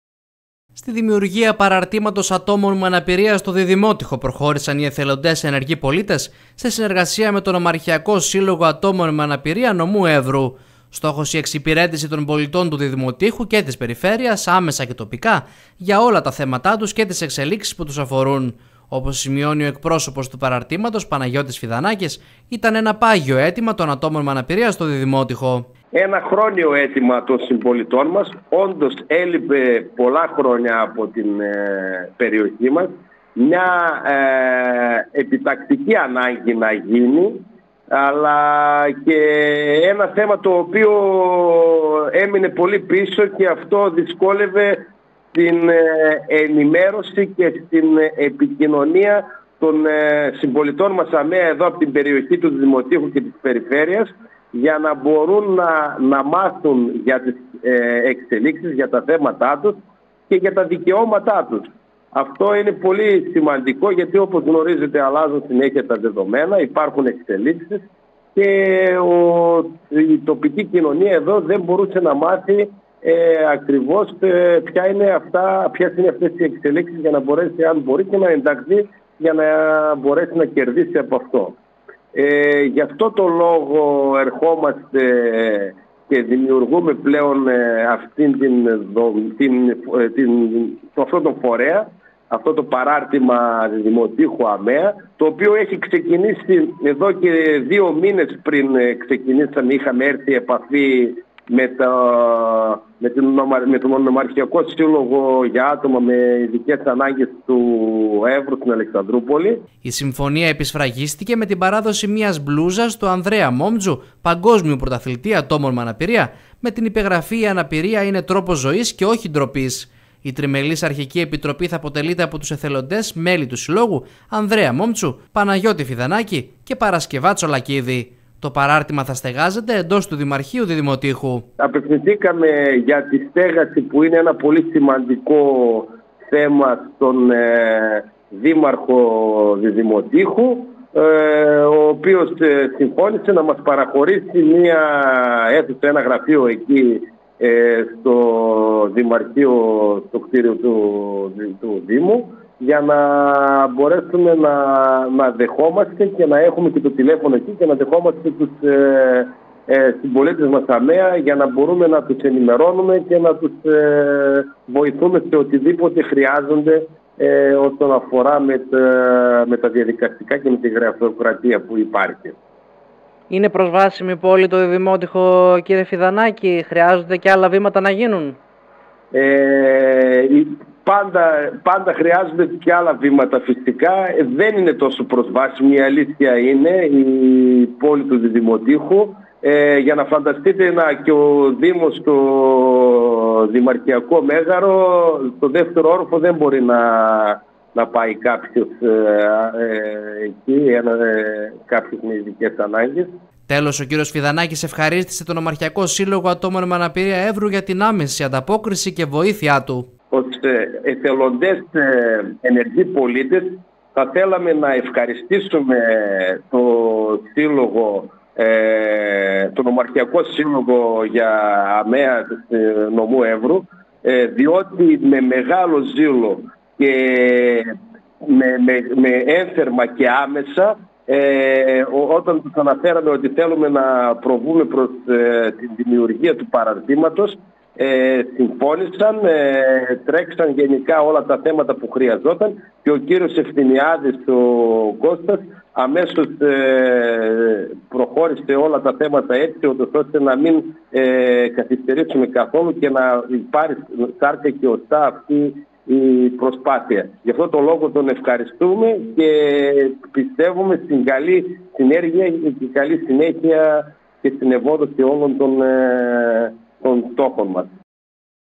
Δημιουργήθηκε παράρτημα ΑμεΑ στο Διδυμότειχο
Παράρτημα ΑμεΑ στο Διδυμότειχο δημιούργησαν οι Εθελοντές Ενεργοί Πολίτες σε συνεργασία με τον Νομαρχιακό Σύλλογο ατόμων με αναπηρία Ν. Έβρου. Πρόκειται για μία σημαντική εξέλιξη που αναμένεται να λύσει αρκετά προβλήματα και αποτελούσε ένα χρόνιο αίτημα των κατοίκων.